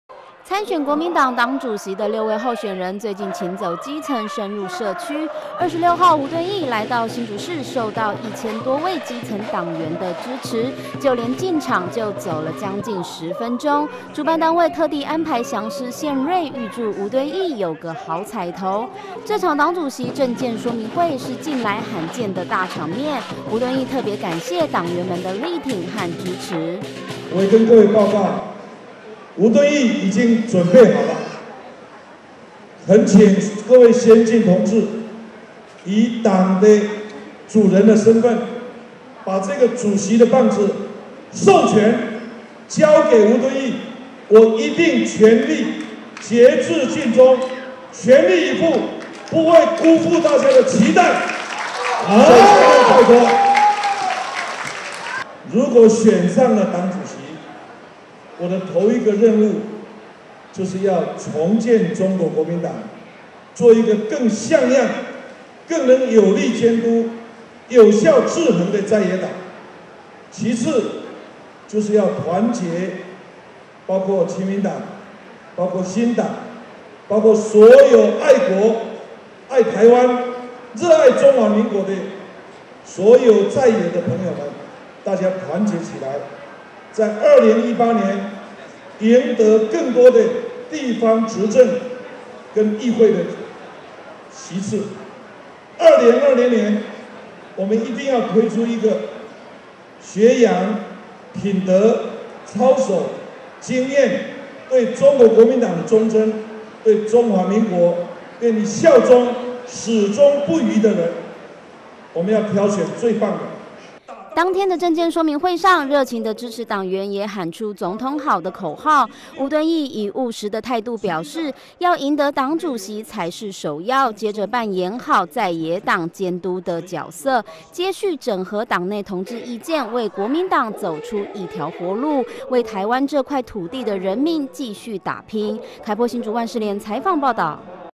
20170426N 吳敦義政見說明會 千位黨員到場支持
國民黨將在5/20進行黨代表以及黨主席的選舉，尤其在黨主席選舉方面有6位候選人角逐黨主席的寶座，候選紛紛勤走基層。4/26號吳敦義也再次走訪新竹市，一千多位基層黨員現身力挺，吳敦義闡述政見，爭取黨員支持贏得勝選。